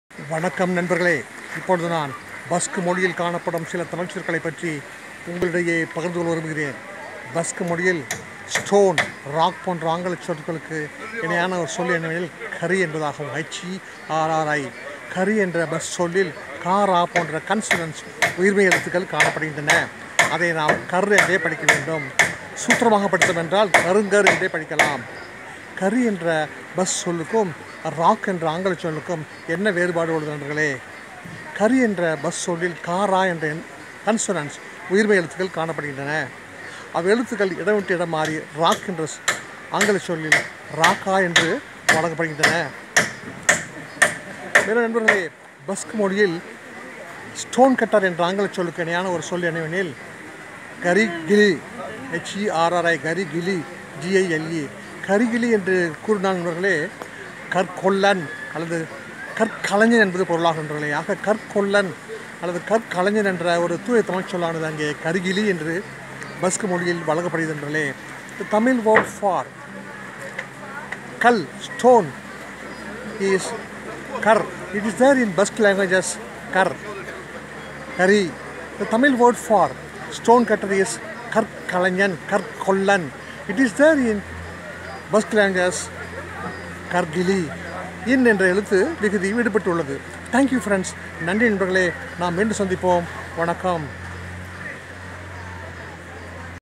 Tamil is the mother of all languages. Basque words.
Stone /  rock  (  herri)    and  stone  cutter  (  herrigile) ( kar  kollan / kar  kalaignan ).